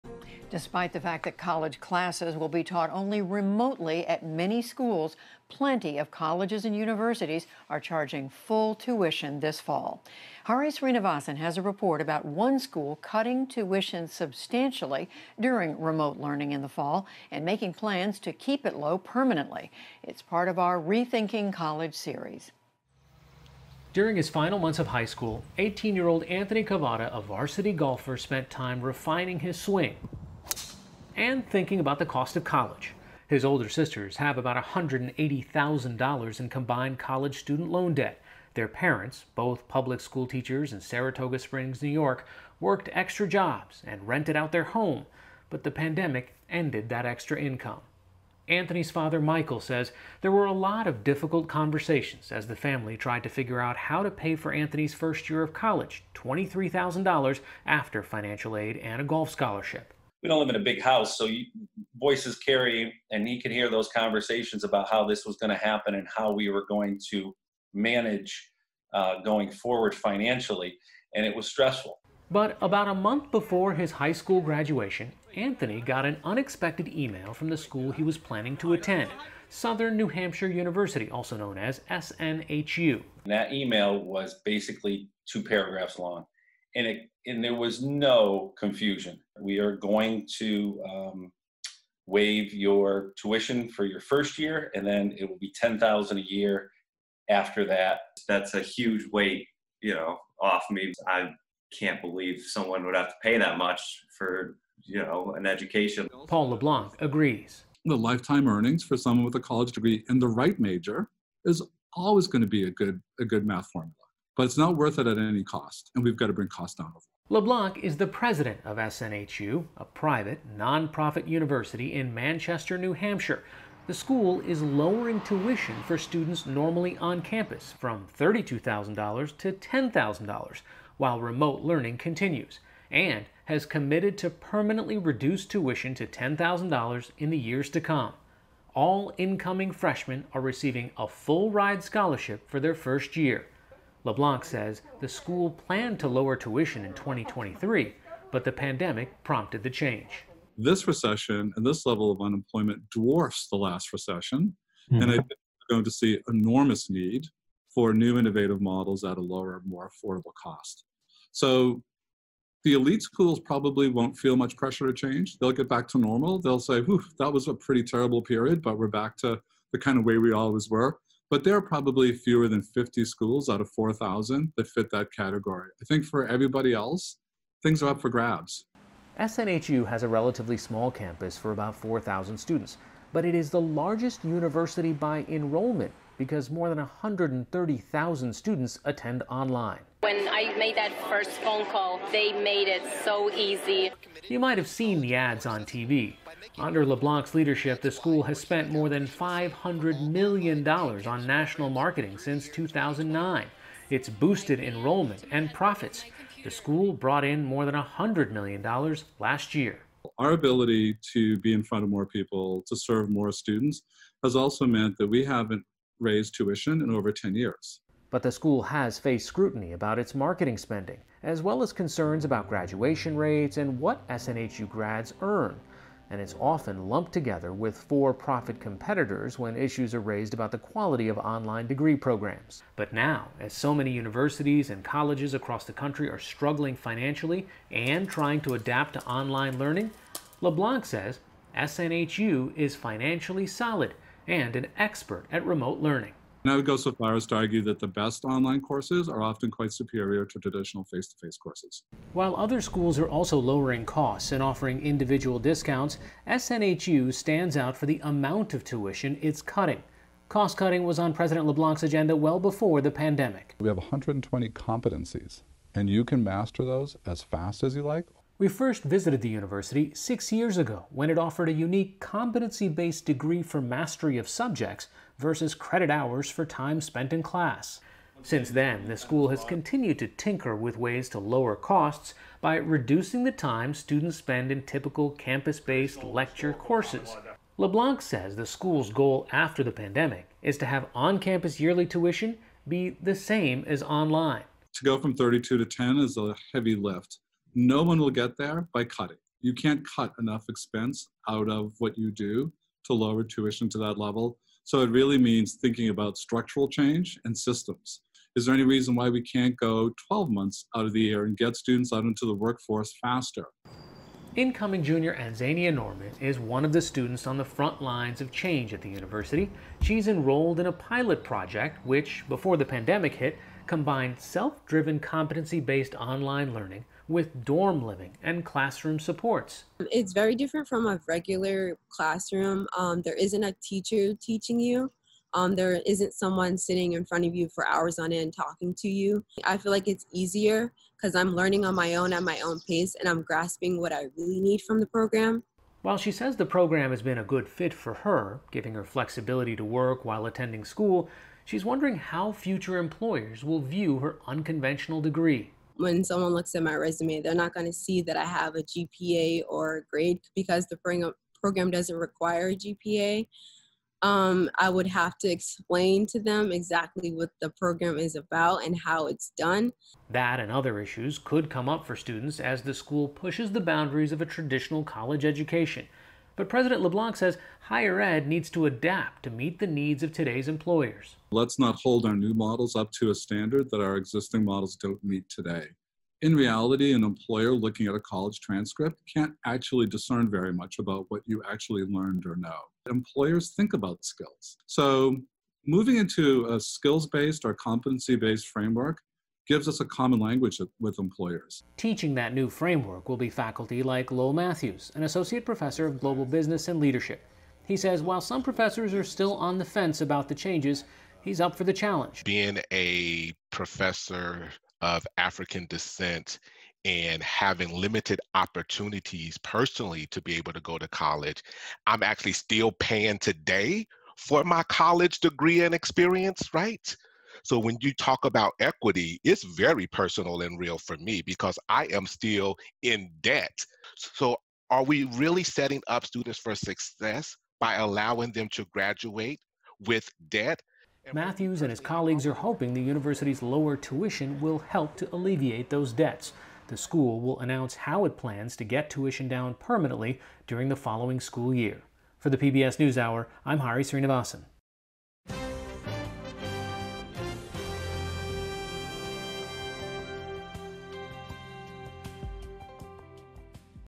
This college lowered tuition due to the pandemic -- and plans not to raise it again
U.S. colleges have struggled with how to conduct the upcoming academic year amid the coronavirus pandemic. With many schools deciding to offer only remote classes but still charging full tuition, the pandemic has added a new urgency to questions about the cost and value of higher education. Hari Sreenivasan reports on one school that is cutting tuition -- and planning to maintain the reduction.